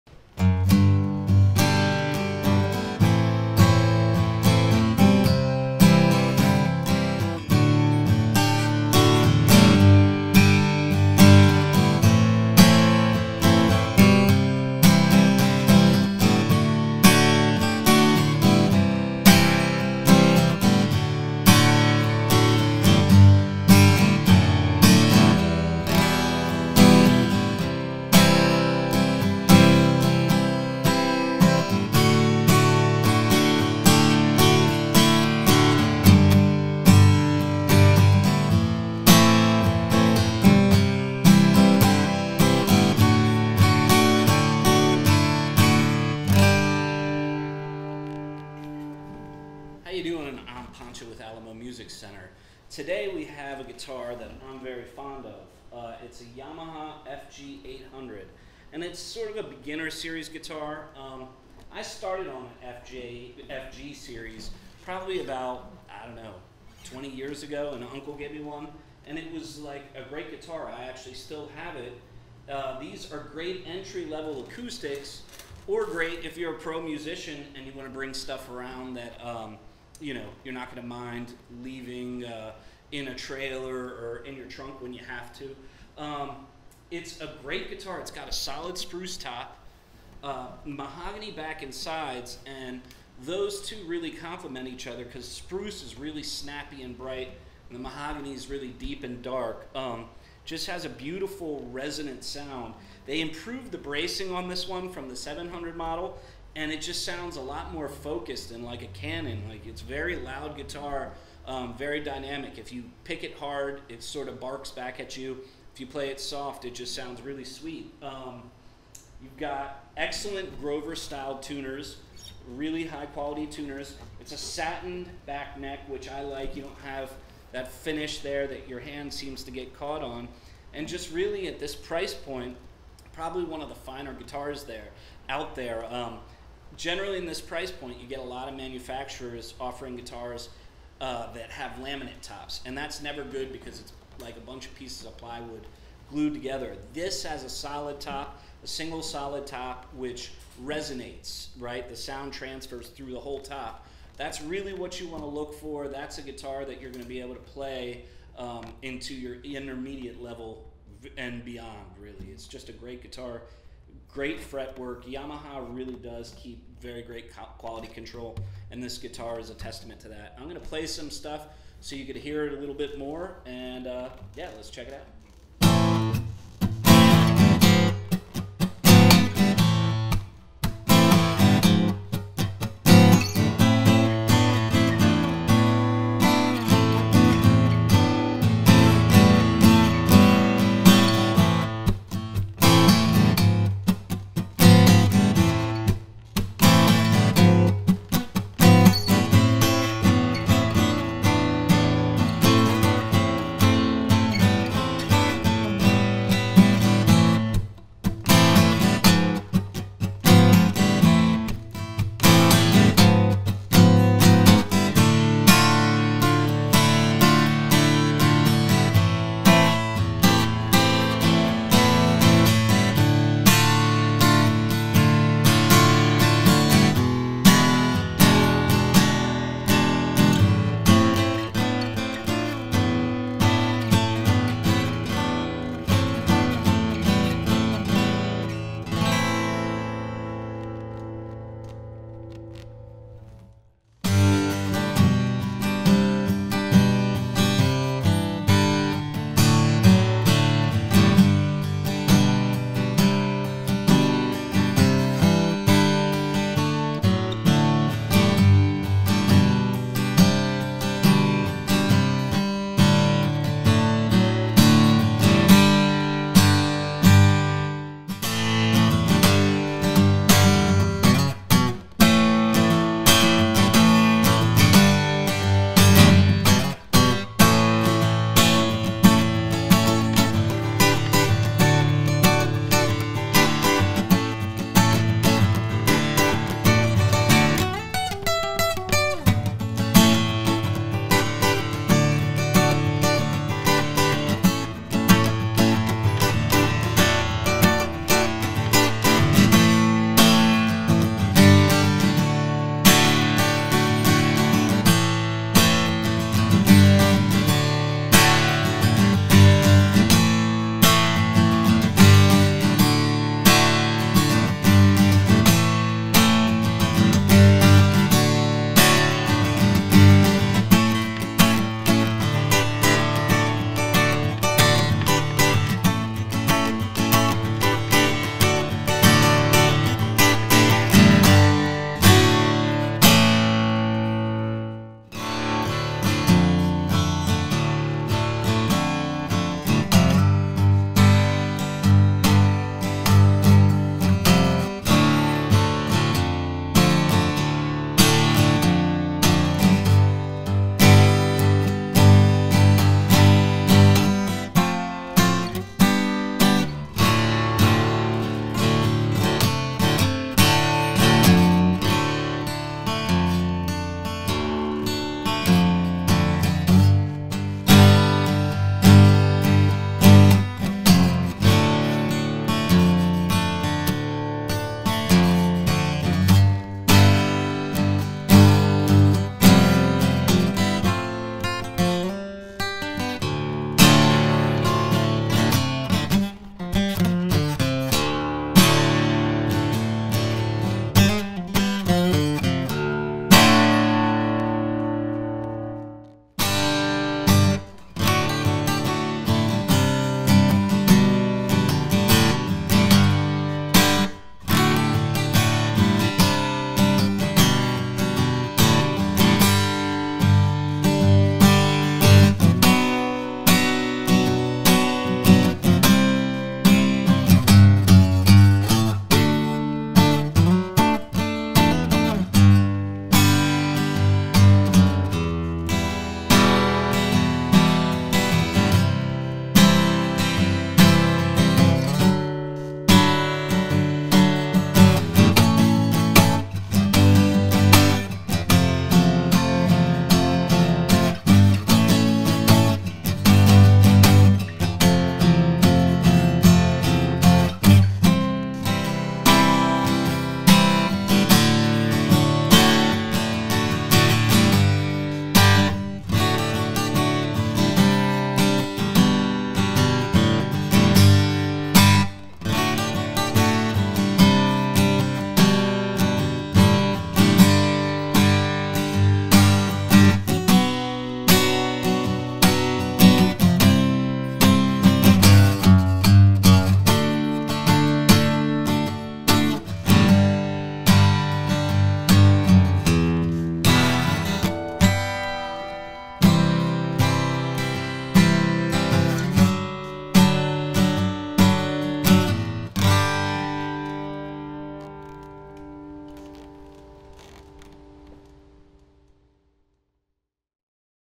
Yamaha FG800 Demo & Review
Pancho Garza reviews the Yamaha FG800.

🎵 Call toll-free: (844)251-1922
🎶 Connect: @alamomusic